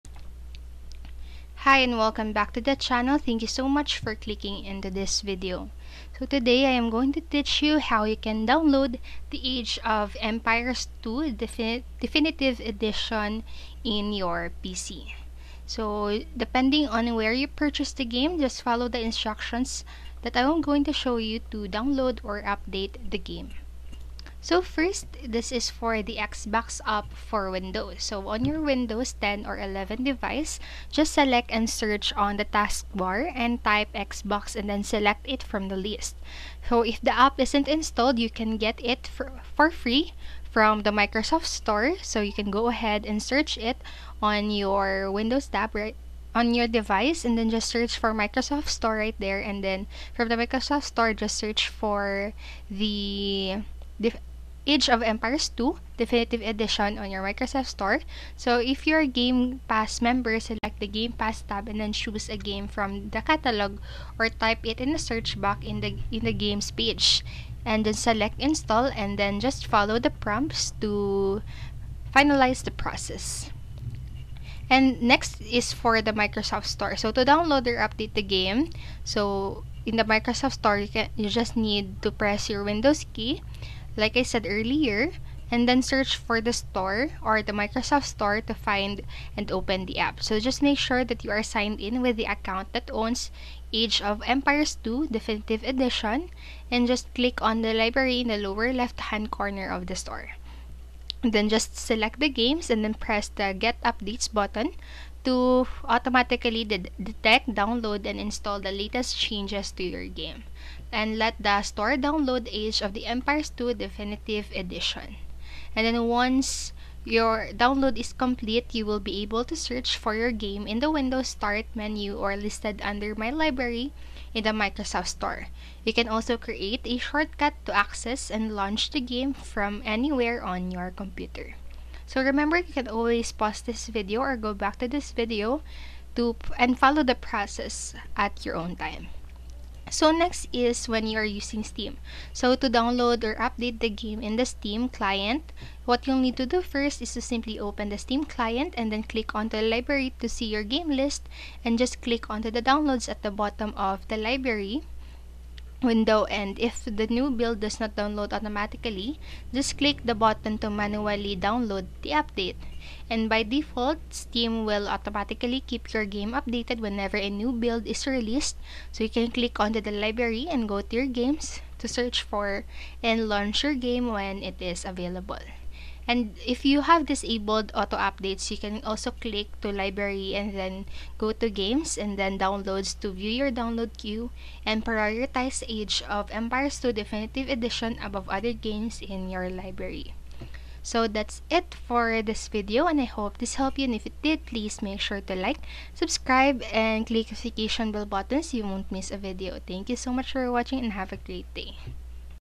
How To Download Age Of Empires 2 Definitive Edition in PC (2025 Guide)
Learn How To Download Age Of Empires 2 Definitive Edition in PC (2023 Guide). This video will show you how to install age of empires 2 definitive edition on pc.To download Age of Empires II: Definitive Edition on PC, follow these steps:

Open the Microsoft Store: Go to the Microsoft Store app on your Windows PC.

Search for Age of Empires II: Definitive Edition: Use the search bar in the Microsoft Store to look for "Age of Empires II: Definitive Edition."

Purchase or Install: If you haven't purchased the game yet, click on the "Buy" button to make the purchase. If you already own the game or if you're eligible for a free install, click on the "Install" button.

Sign in with Microsoft Account: If prompted, sign in with your Microsoft account to complete the purchase or installation process.

Download and Install: The game will now start downloading and installing automatically. The download time may vary depending on your internet connection speed.

Launch the Game: Once the download and installation are complete, you can launch Age of Empires II: Definitive Edition from the Start menu or the Microsoft Store app.

Please ensure that your PC meets the system requirements for Age of Empires II: Definitive Edition to run smoothly. The minimum system requirements are:

OS: Windows 10 64-bit
Processor: Intel Core 2 Duo or AMD Athlon 64x2 5600+
Memory: 4 GB RAM
Graphics: NVIDIA GeForce GT 420 or ATI Radeon HD 6850 or Intel HD Graphics 3000 or better
DirectX: Version 11
Network: Broadband Internet connection
Storage: 30 GB available space
Note: Age of Empires II: Definitive Edition is available through the Microsoft Store, and you can only install and play it on Windows 10 PCs. It is not available for other operating systems like macOS or Linux.